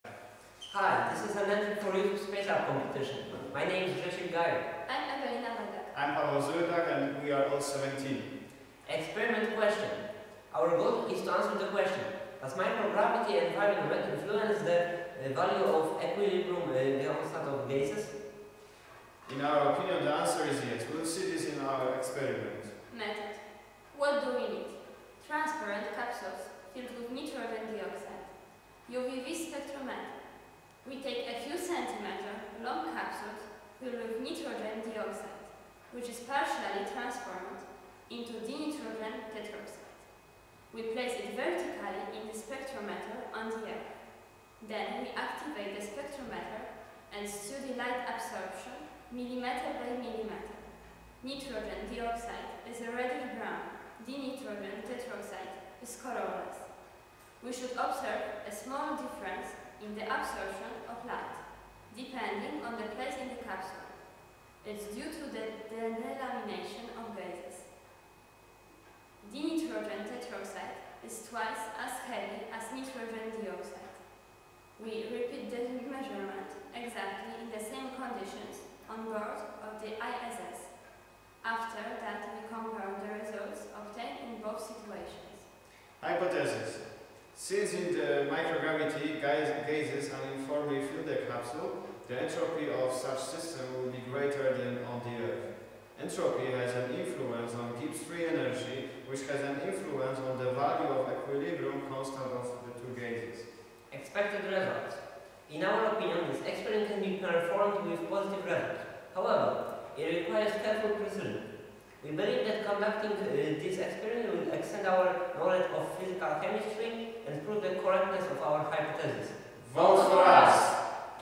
YouTube Space Lab Entry 2 - Physics Experiment
Entry 2
Physics Experiment
Hi! This is an entry for YouTube Space Lab competition. My name's Grzesiek Gajoch.
I'm Ewelina Badak.
I'm Paweł Złydach. We are all seventeen.
Experiment Question
Our goal is to answer the question "Does microgravity environment influence the value of the equilibrium constant of gases?".
In our opinion, the answer is "yes". We'll see this in our experiment.
Method
What do we need?
• Transparent capsules filled with nitrogen dioxide
• UV-VIS spectrometer
We take a few-centimeter long capsule filled with nitrogen dioxide (NO2),  which is partially transformed into dinitrogen tetroxide (N204). We place it vertically in the spectrometer on the Earth. Then we activate the  spectrometer and study light absorption millimeter by millimeter. Nitrogen dioxide is a reddish-brown, dinitrogen teroxide is colorless. We should observe a small difference in the absorption of light, depending on the place in the capsule. It is due to the delamination of gases. Dinitrogen teroxide is twice as heavy as nitrogen dioxide. We repeat this measurement exactly in the same conditions on board of the ISS. After that we compare the results obtained in both situations.
Hypothesis:
Since, In microgravity gases uniformly fill the capsule, the entropy of such system will be greater than on the Earth. Entropy has an influence on Gibbs free energy, which has an influence on the value of equilibrium constant of the two gases.
Expected Results
In our opinion, this experiment can be performed with positive results. However, it requires careful precision. We believe that conducting this experiment will extend our knowledge of Physical chemistry, and prove the correctness of our hypothesis.
VOTE FOR US!!!